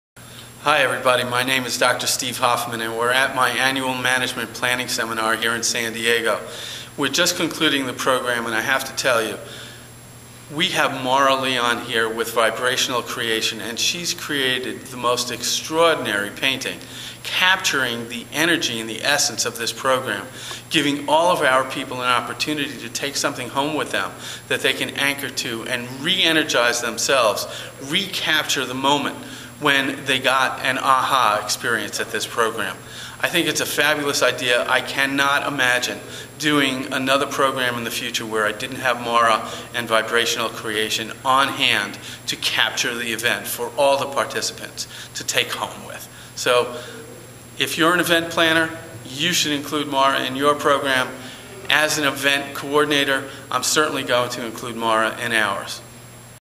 Vibrational Creation testimonial Dr. Steve Hoffman.wmv
Testimonial for Maura Leon Vibrational Creation - Vibrational painting created at a Live Seminar Event in San Diego. This was a two day event hosted by Dr. Steve Hoffman. It was a powerful and transformational event.